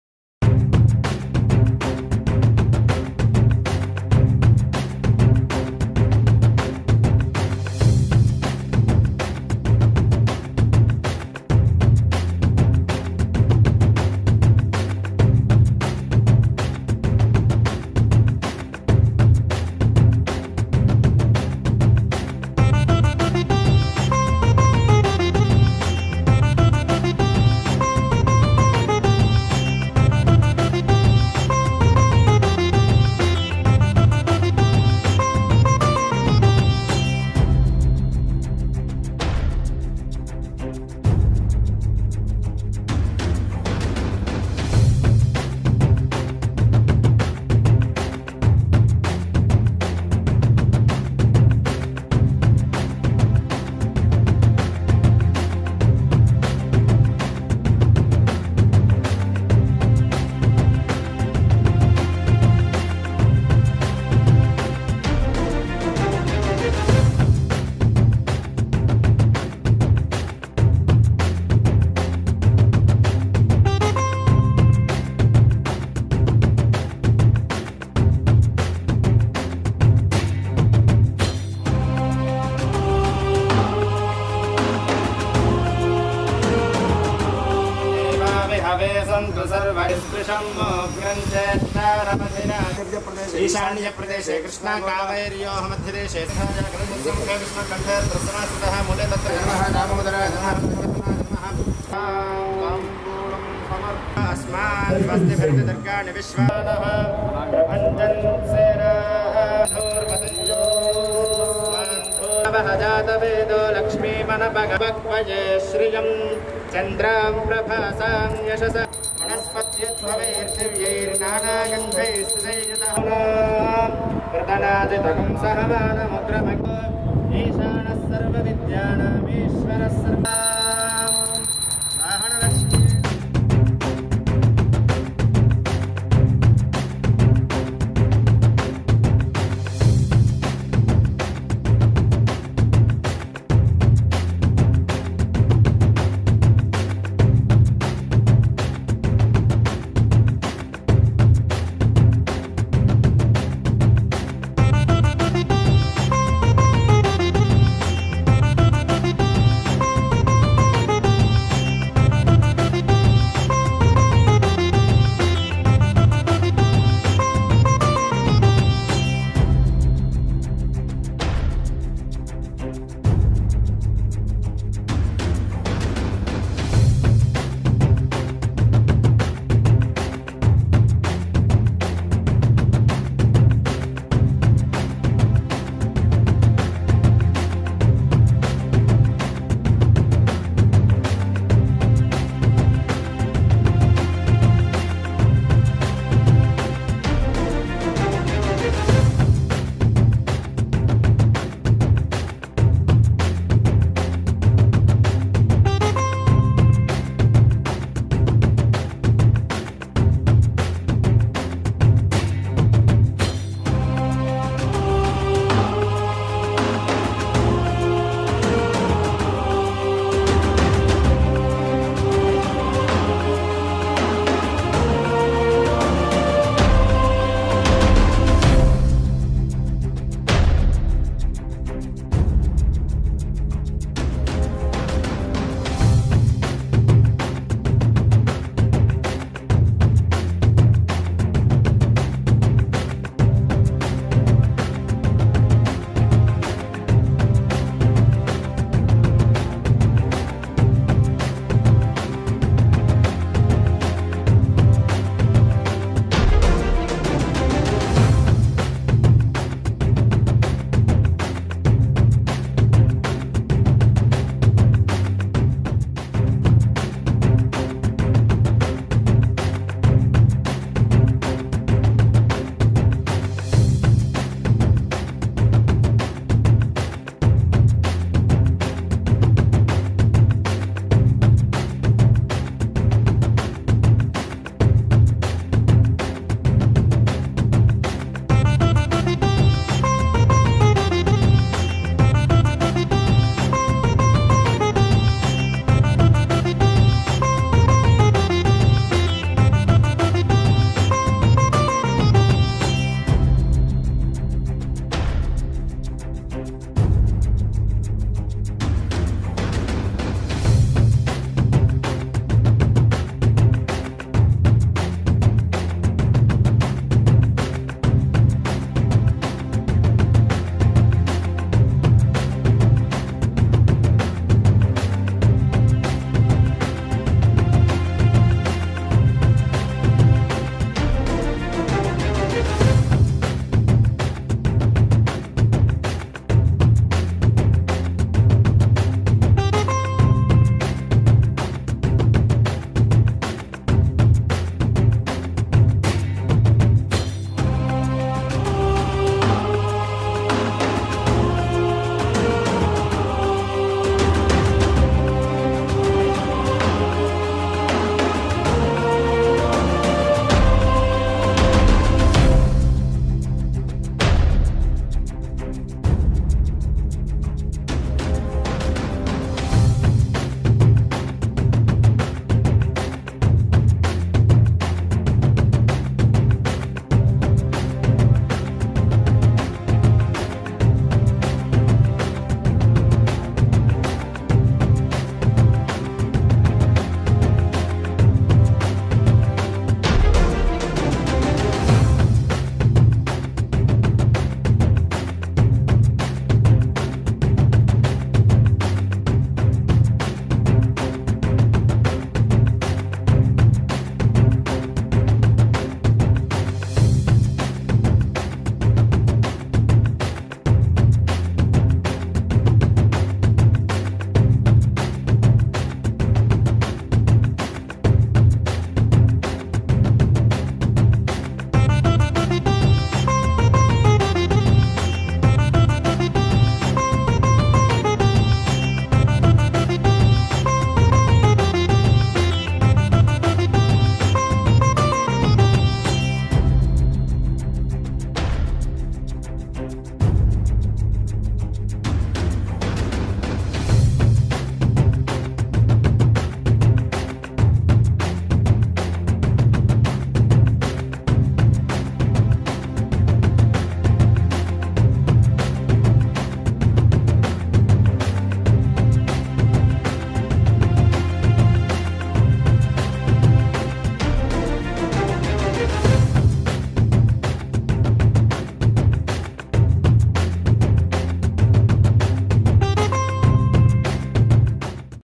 రాష్ట్ర యాత్రకు సిద్దమైన పవన్ కాన్వాయ్ | Pawan Kalyan Convoy New Cars Completes Pooja Ceremony
Janasena Chief Konidela Pawan Kalyan bought 8 new convoys for Janasena election campaign in AP and completed their pooja ceremony in Mangalagiri.